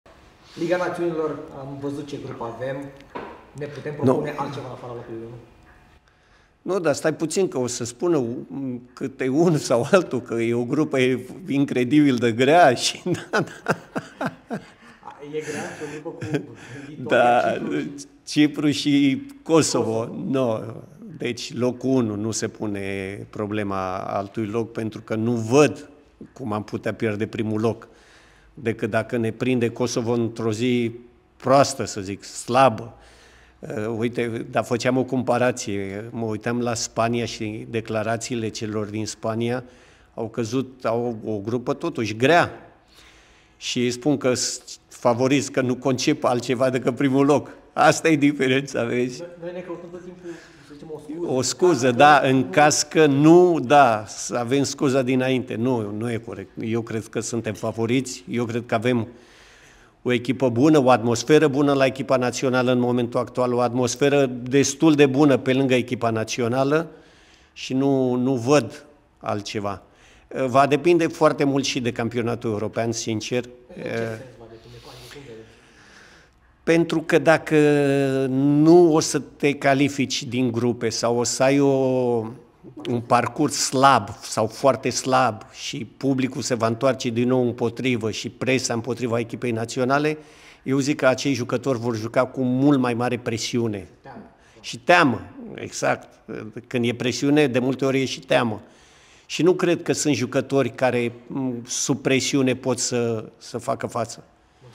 Gică Craioveanu, despre grupa României din Liga Națiunilor: ”Va conta mult și ce vom face acolo”
Gică Craioveanu a explicat într-un interviu pentru iAMsport.ro ce șanse are naționala României în grupa din Liga Națiunilor.